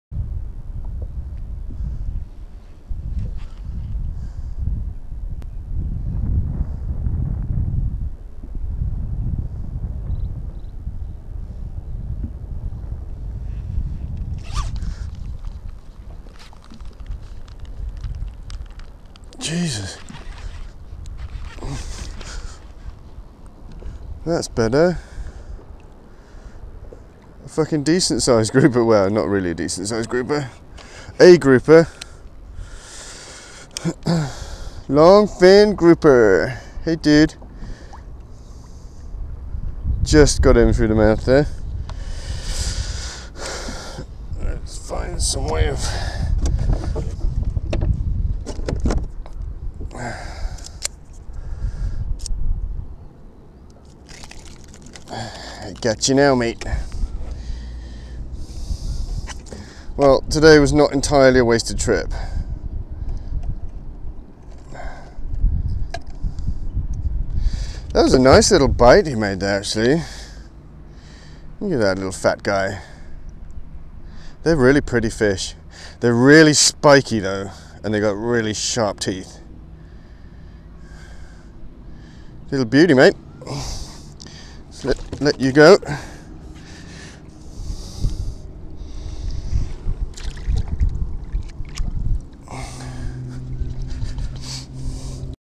Little Longfin Grouper off Koh Samui
4 sessions with no fish. so i went kayak fishing for Grouper, who never disappoint!